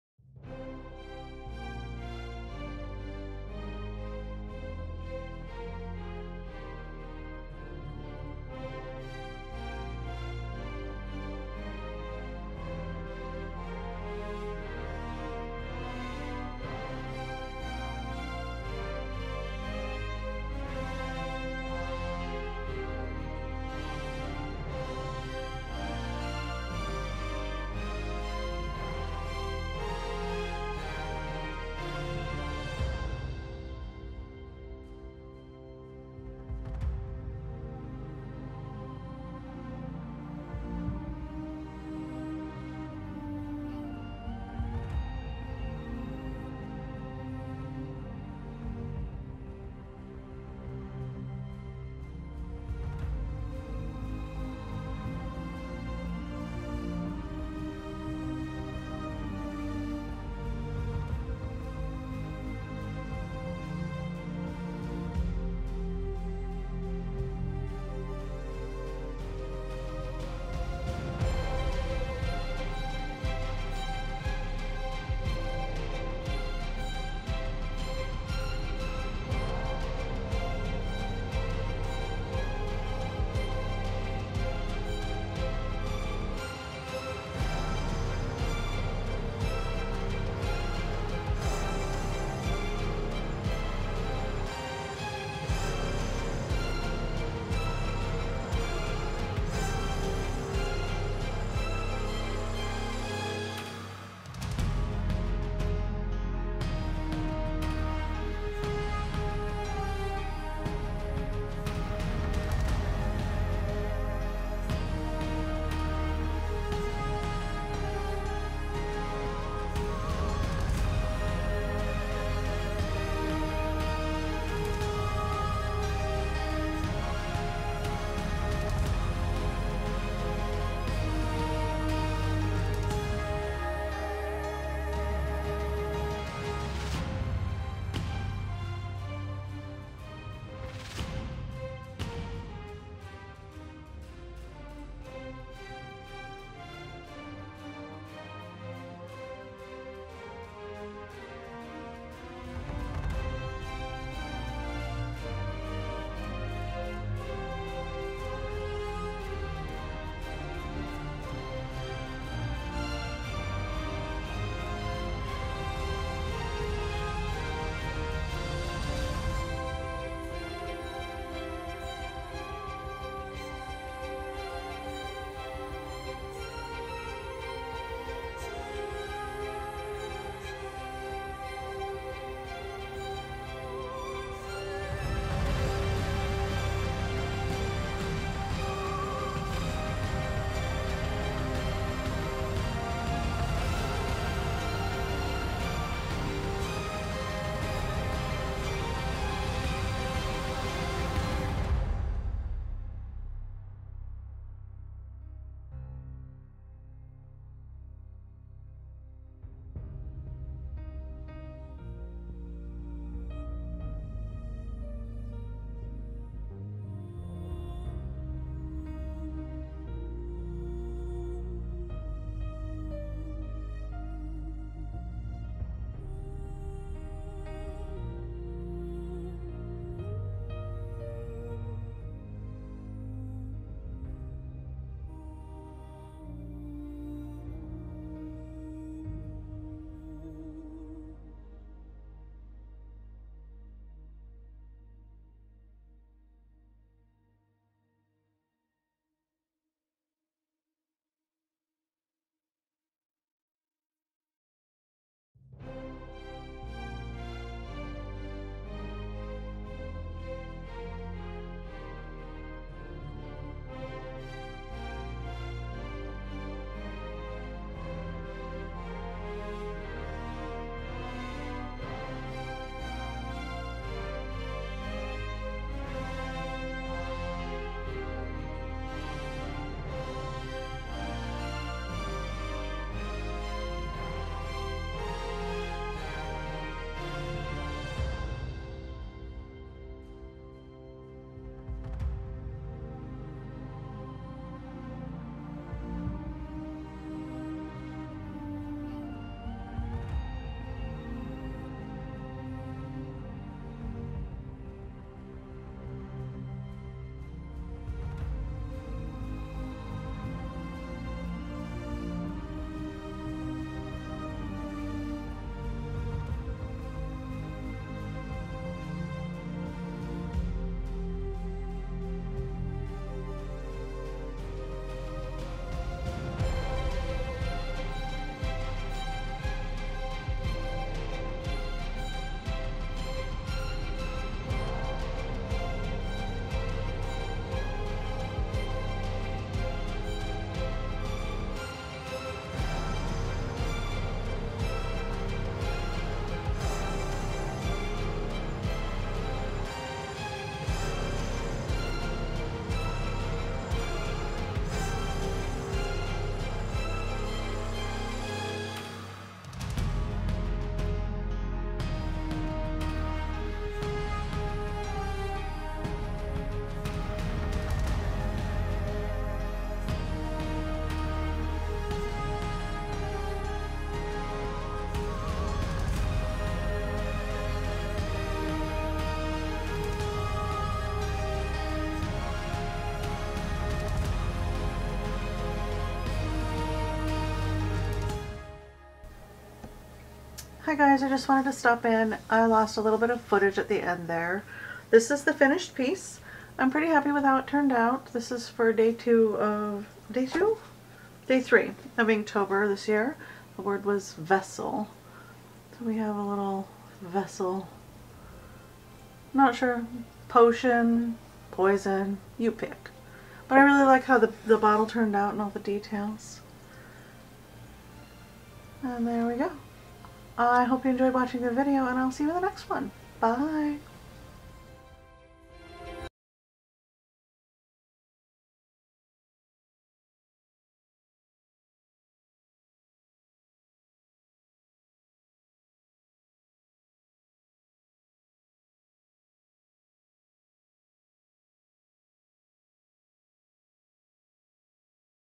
Inktober 2021 Day 3 - Vessel - Time Lapse
Time Lapse of Inktober 2021 day 3- prompt Vessel.

Supplies
Bristol paper
M Graham Watercolors
Arrtx-ALP Grayscale Marker Set
Ink Pen .05